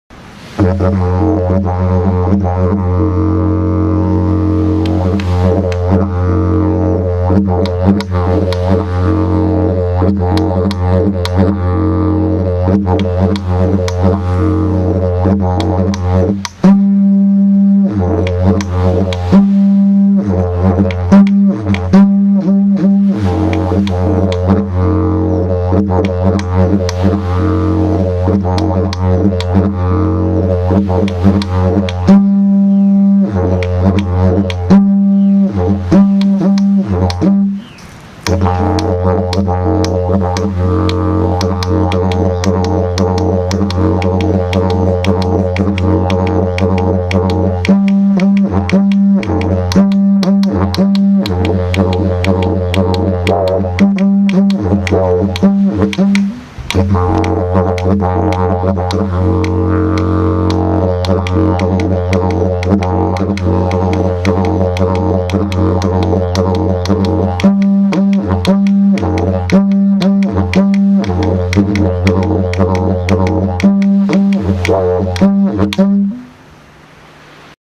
Stunning Mirarra' F/G yidaki, played by Nicky Yunupingu
This is a favourite instrument of both Nicky Dharnmirrwuy and Dhapan Yunupingu, who each independently commented on the outstanding qualities of this yidaki. The purity of its fundamental note and the ease of the overtone are noteworthy, as is the immense bass response of this wonder-stick. This yidaki represents the cream of Mirarra's output... they don't get any better than this!

The instrument is played here by current Yothu Yindi yidaki player Nicky Dharnmirrwuy Yunupingu.